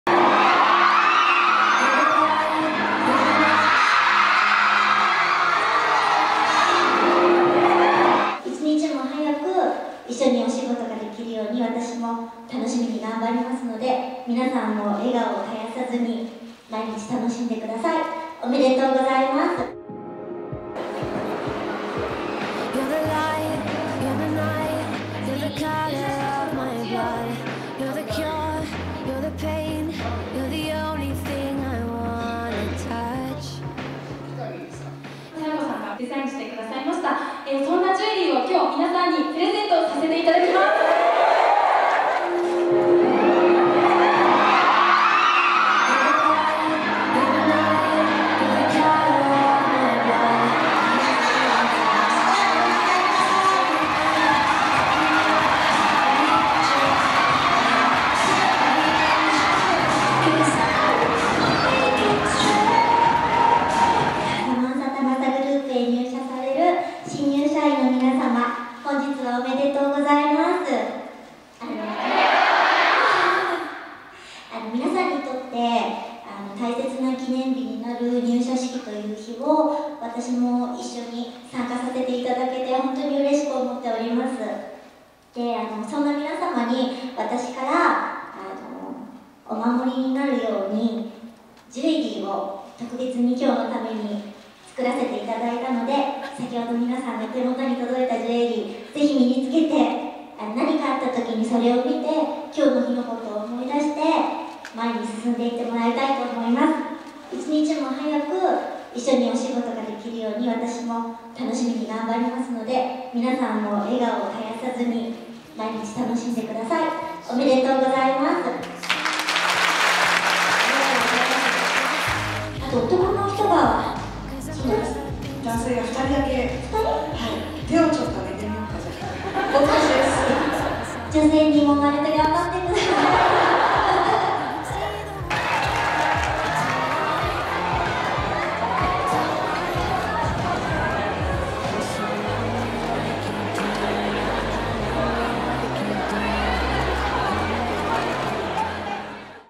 紗栄子、サマンサ入社式にサプライズ登場 涙する新入社員も
モデルでタレントの紗栄子が1日、都内で行われたサマンサタバサグループの入社式にサプライズ登場。司会の呼び込みで華やかなピンクのワンピースに身を包んだ紗栄子が会場に姿をみせると、新入社員525人は大興奮。紗栄子は「皆さんと一日も早く、一緒にお仕事ができるように私も楽しみに頑張りますので、皆さんも笑顔を絶やさず毎日頑張ってください」と新たな門出を祝福した。

　入社式では同グループのジュエリーブランド『Samantha Tiara』においてプロモーションモデルとコラボラインのデザイナーを務める紗栄子のオリジナルジュエリーのブレスレットを新入社員全員にプレゼント。「皆さまにお守りとしてジュエリーを作らせていただいたので、私からなにかあった時に、それを見てきょうの日のことを思い出し、前に進んでいってほしいてと思います」と願いを込めた。

　さらにサプライズは続き、この日誕生日を迎えた新入社員1人が壇上に呼ばれ、紗栄子本人からブレスレットを装着。感極まる新入社員の涙を紗栄子が直接、手でぬぐうと再び大きな歓声が。また、会場内に2人のみ参加した男性新入社員を見つけた紗栄子は「女性にもまれて頑張ってください！」とエールを送っていた。